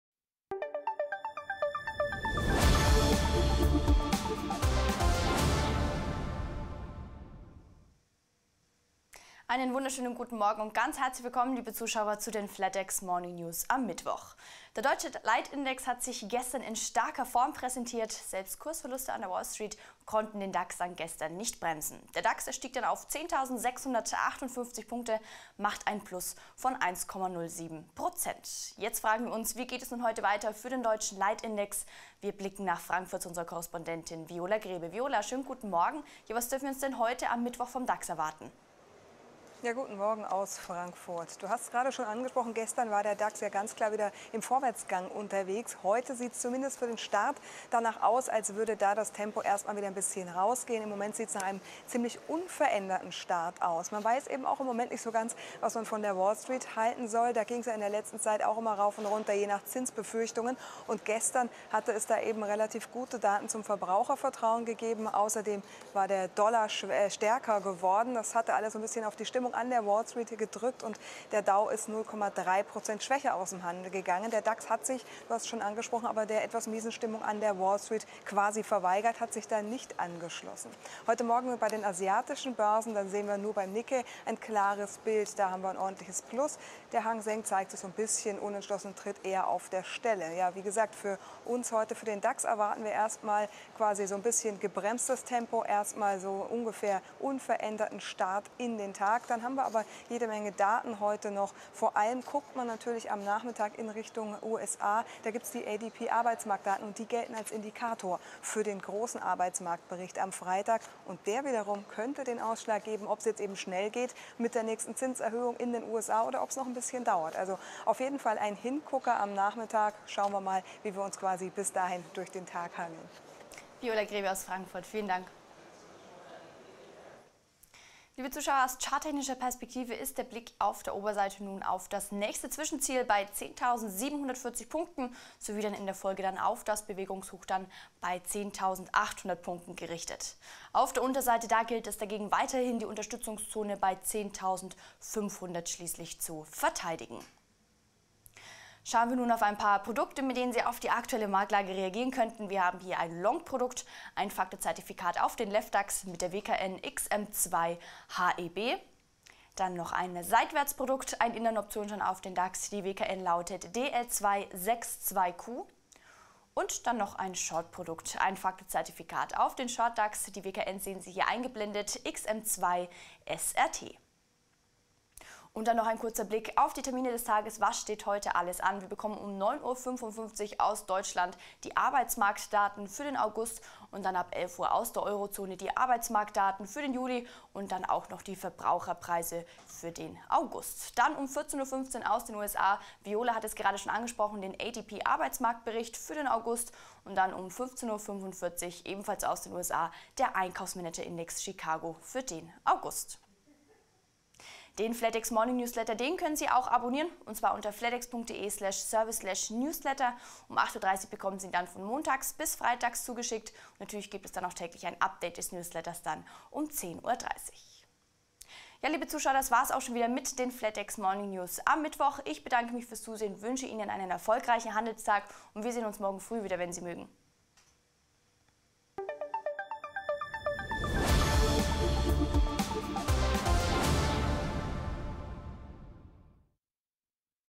Flatex Morning News: Dax heute unverändert erwartet
Der deutsche Leitindex hat sich gestern in starker Form präsentiert. Selbst Kursverluste an der Wall Street konnten den DAX nicht bremsen. Der DAX stieg auf 10.658 Punkte. Das ist ein Plus von 1,07 Prozent.